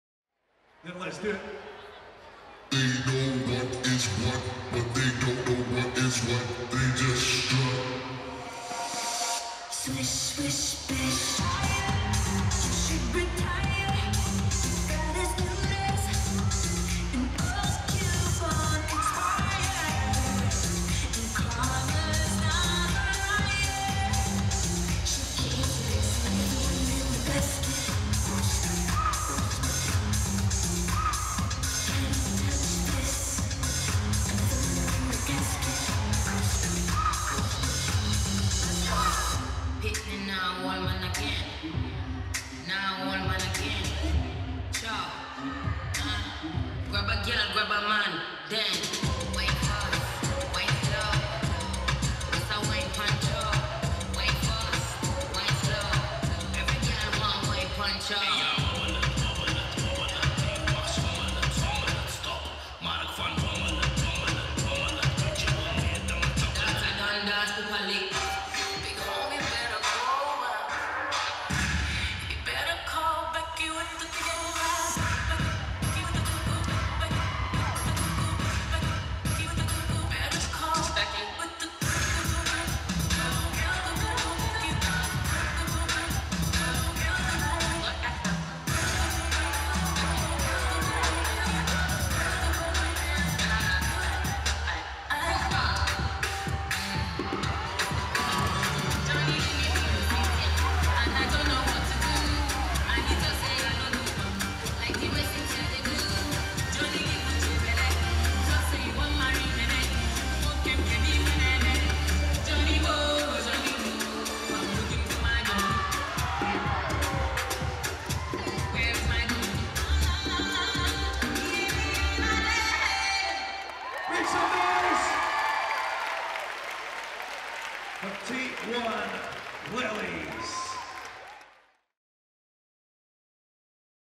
TAKE 1 LITTLIES - BDO North West Street Dance Championships 2017
Team performance from the BDO North West Street Dance Championships 2017 at Wright Robinson College in Manchester.